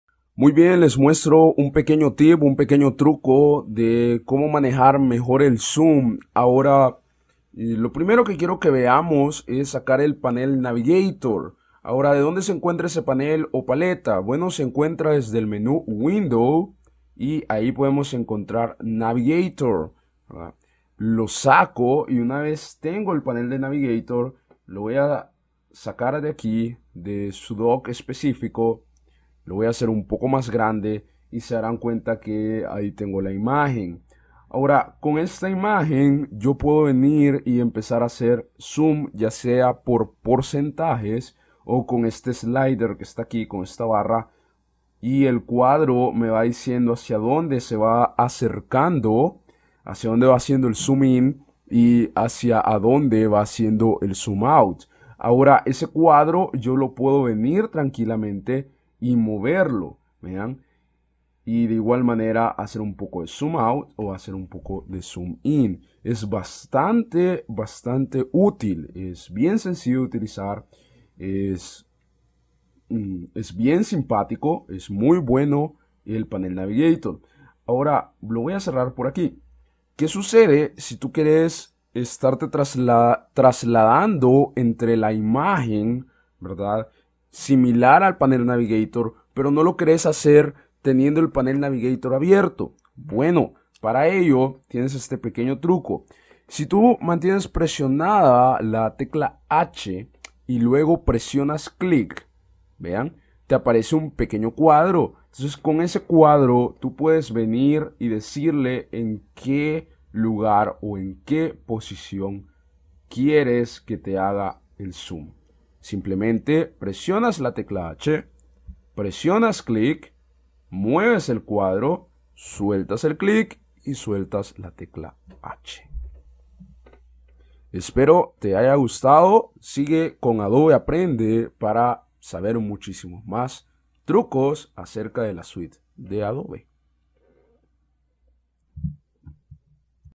Un pequeño truco de como hacer zoom en Photoshop
Este es un truco que con un buena aceleradora de video este truco se vuelve indispensable a la hora de trabajar con el zoom de Photoshop CS4